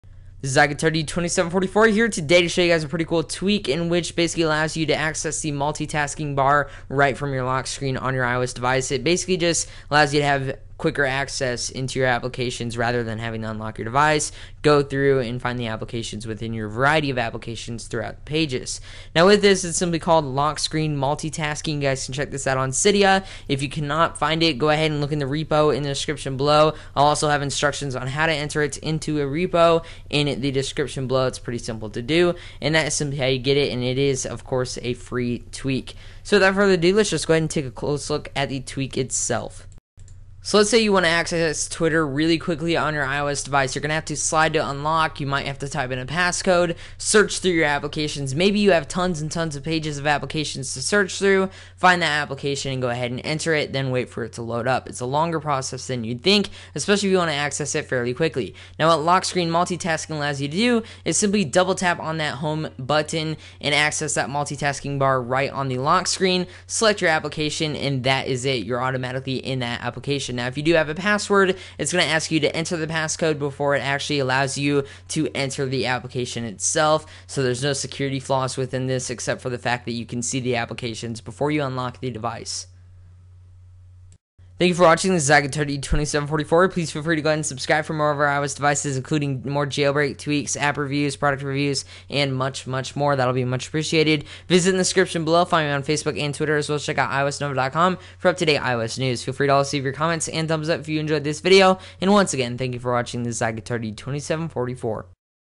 LockScreen Multitasking Tweak for iPhone, iPod Touch, and iPad
LockScreen Multitasking is a new tweak that allows you to access applications more quickly from the lockscreen of your device. It saves you the hassle of having to search through your pages and folders to find the app. Simply double tap and access your application although, I don't call this multitasking. I prefer to call it fast app switcher. Please also comment and thumbs up.

Go to Cydia - Manage - Sources - Edit - Add.